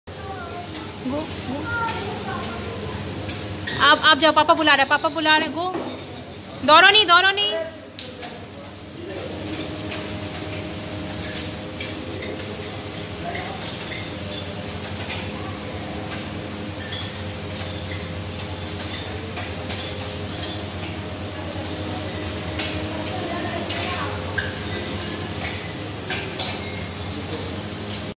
HapiTavi eating in Haridwar
HapiTavi is eating in Haridwar in Indian style.